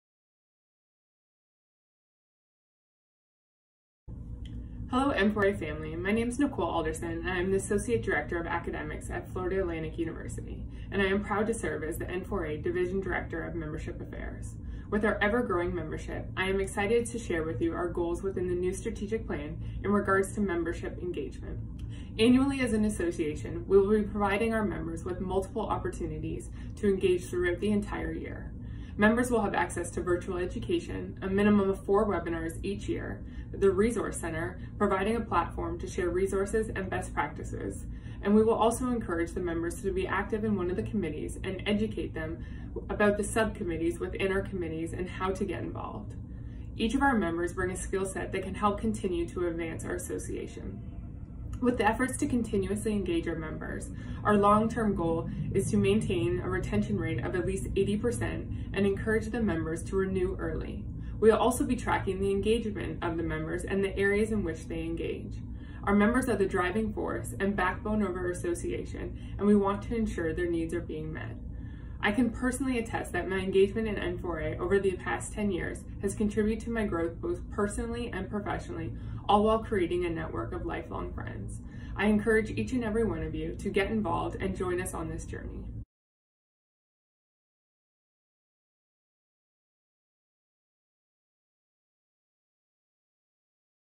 N4A Strategic Plan, Membership Services
In this video, Nicole Alderson, Division Director for Membership Affairs, will discuss the Membership Engagement portion of N4A's Strategic Plan.  Nicole will highlight both the short-term and long-term initiatives outlined in the plan.

Nicole Alderson, Divison Director for Membership Affairs, Associate Director at Florida Atlantic University.

Facbook:  @N4ANACDA